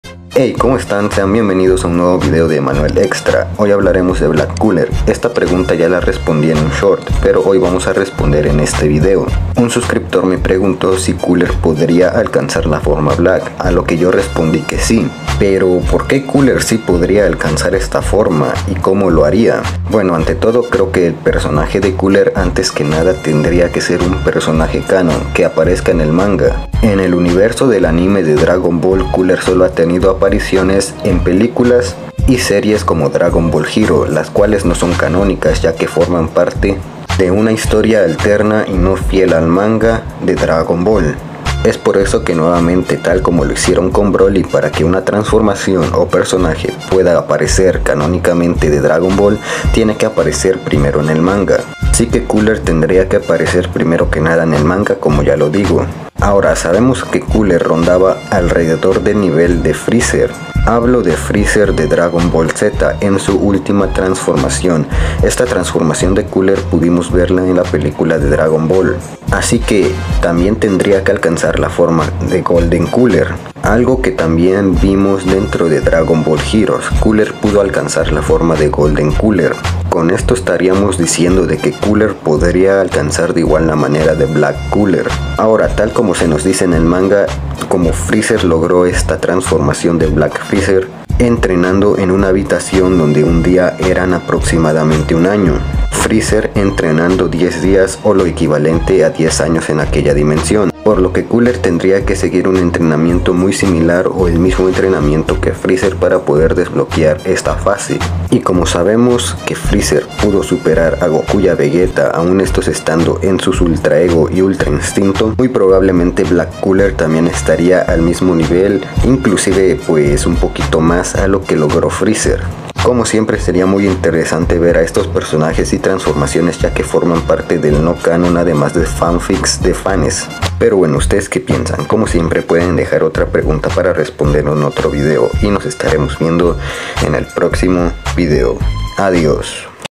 Cooler podría alcanzar el modo Black-Dragonball
en este video veremos si el personaje de Cooler podría alcanzar el modo de Balck Cooler en Dragonball